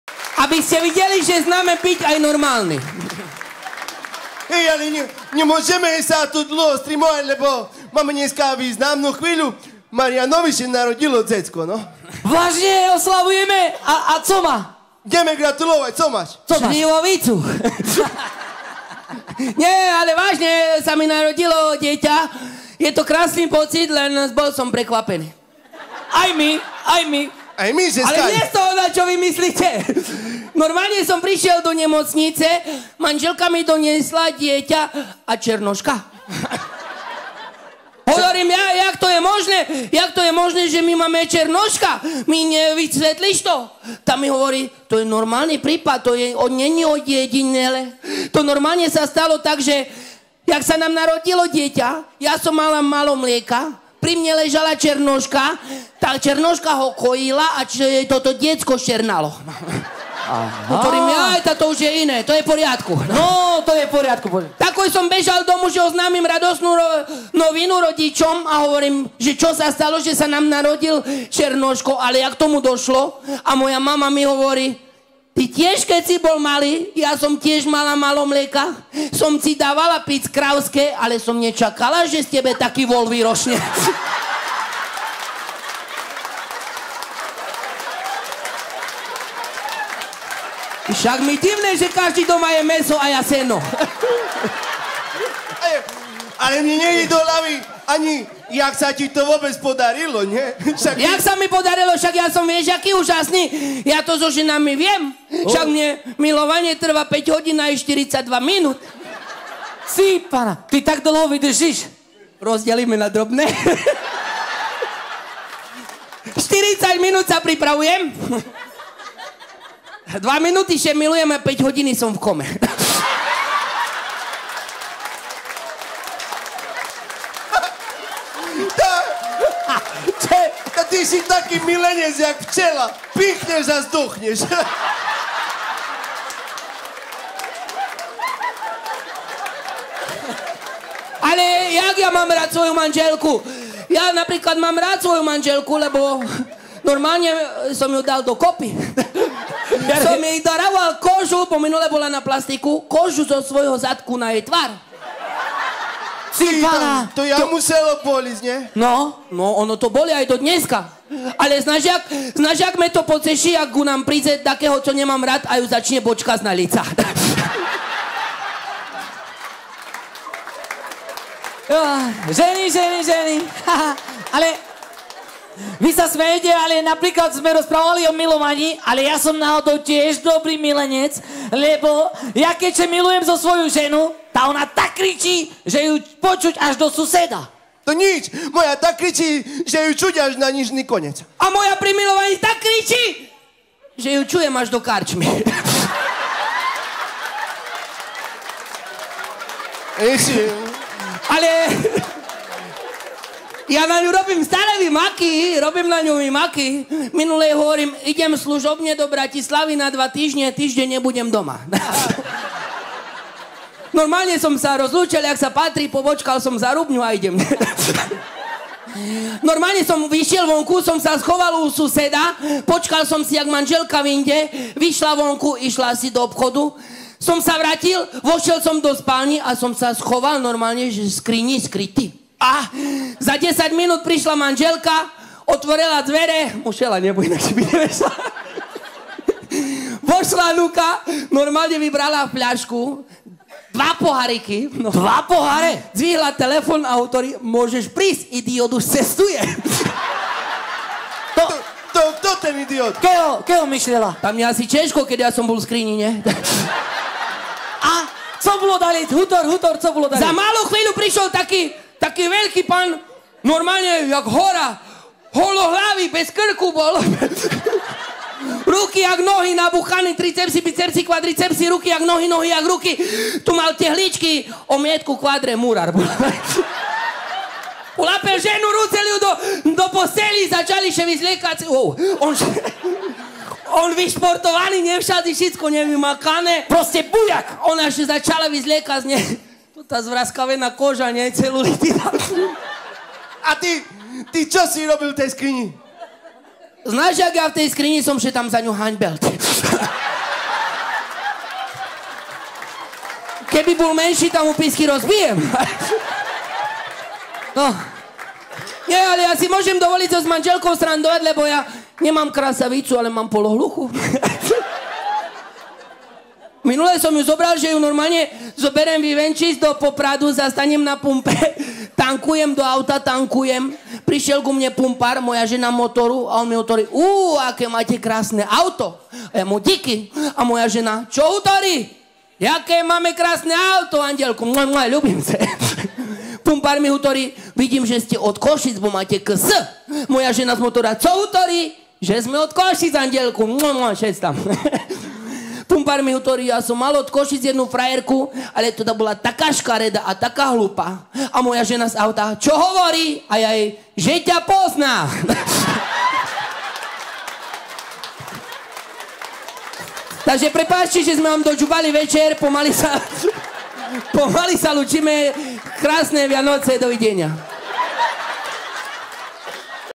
Ščamba: Humorné scénky 3
Pesničky z kasína 26 z klubu Blue Note v Novom Meste nad Váhom
Účinkujú: HS Rytmus z Novej Bošáce, Zdeněk Krásný, Ščamba, Oľga Baričičová, Silvia a Peter Klimentovci, Duo Jamaha